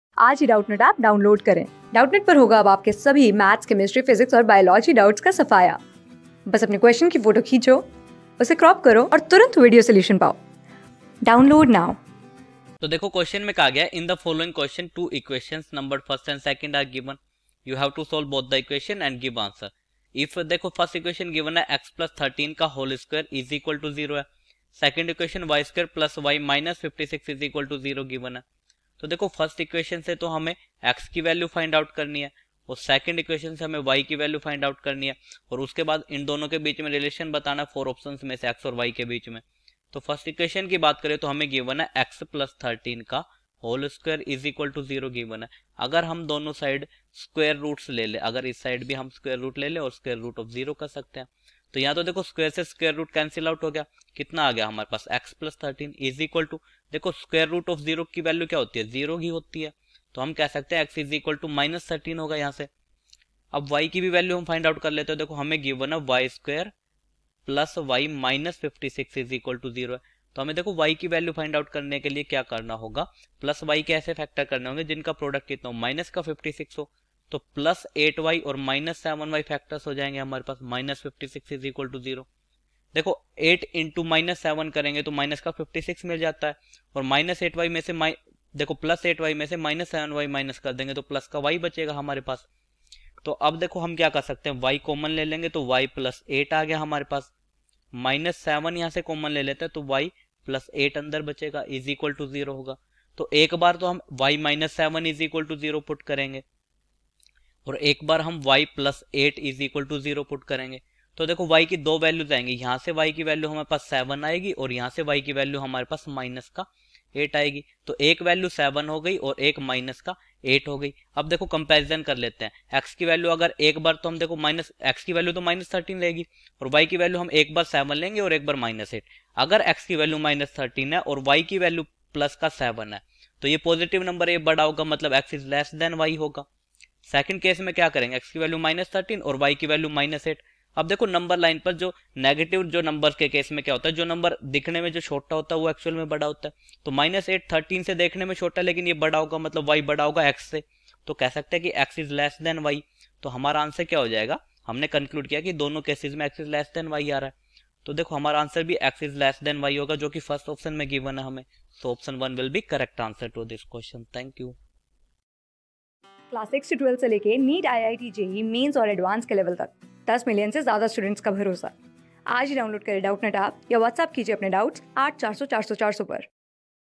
In the following questions, two equations numbered I and II are given. You have to solve both th...
In the following questions, two equations numbered I and II are given. You have to solve both the equations and Give answer If (x+13)^2=0 y^2+y-56=0
Class: 14
Subject: MATHS
Chapter: SBI PO PHASE - I (PRELIMINARY) EXAM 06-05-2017
Board:GOVT

👉 Have Any Query? Ask Us.